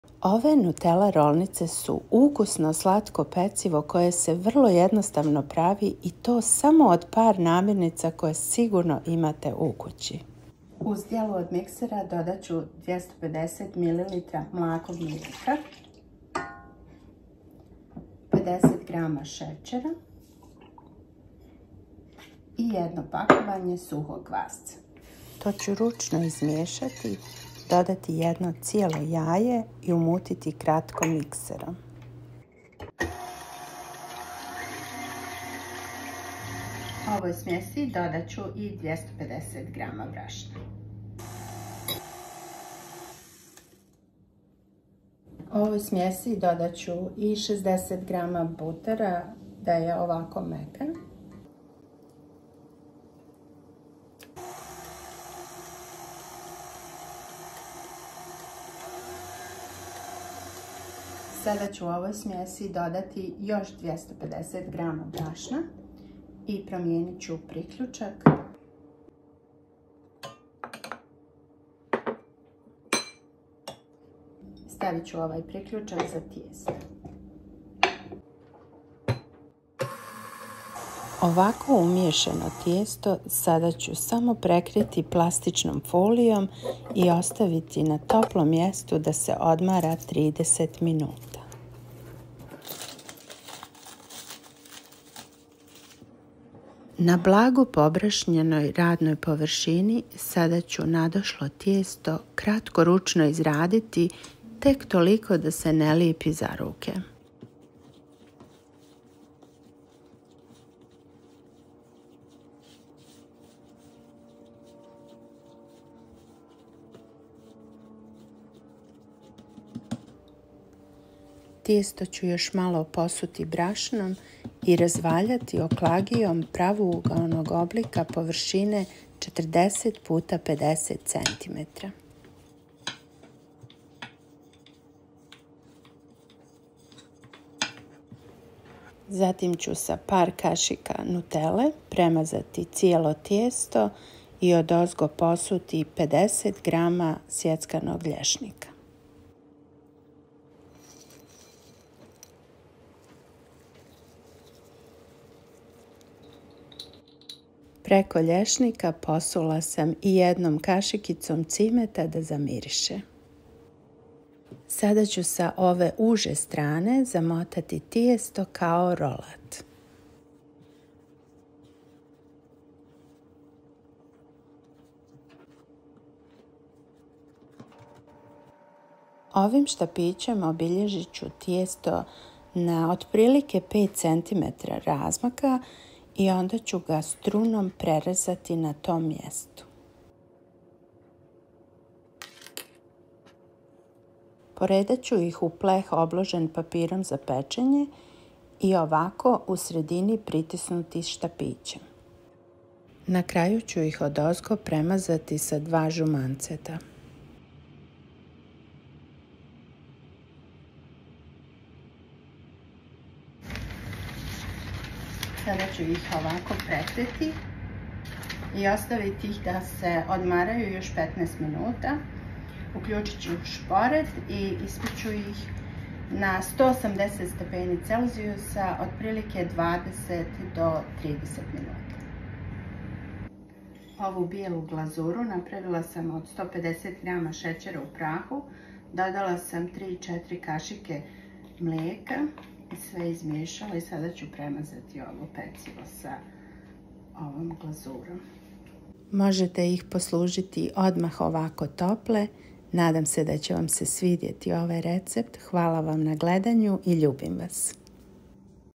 Nutella Rolls | Ukusno Mekano Slatko Pecivo
Ukusno mekano slatko pecivo koje možete filovati po svom ukusu, raznim kremama, džemovima i slično… ja sam se ovoga puta odlučila za čokoladnu varijantu. Jednostavna i idealna poslastica za cijelu porodicu!
Nadam se, da će vam se dopasti ovaj recept. Pišite mi u komentarima kako vam se sviđa, slobodno podijelite recept sa vašim prijateljima i familijom!!🤎

⬇️⬇️⬇️ RECEPT JE ISPOD ⬇️⬇️⬇️

📋 Potrebne Namirnice:
* 250ml mlakog mlijeka
* 50g šećera
* 1 pak suhog kvasca
* 1 jaje
* 500g brašna
* 60g putera (sobne temperature)
bijela glazura:
* 150g šećera u prahu
* 3-4 kašike mlijeka
Prijatno!

🔔PRIJAVITE se na moj kanal ​⁠  @GamelaSweetArt
👉 zapratite me i na:

Delicious soft sweet pastry that you can fill according to your taste, various creams, jams... this time I decided on the chocolate version. A simple and ideal treat for the whole family!
Hopefully you'll like this recipe, if so please don't forget to click the like button👍🏼 and share this recipe with your friends and family!🤎

⬇️⬇️⬇️ THE RECIPE IS BELOW ⬇️⬇️⬇️

📋 Necessary Ingredients:
* 250ml warm milk
* 50g sugar
* 1 pack of dry yeast
* 1 egg
* 500g of flour
* 60g of butter
white glaze
* 150g powdered sugar
* 3-4 tbsp of milk

🔔 Remember to turn on your notifications so you never miss a new video! @GamelaSweetArt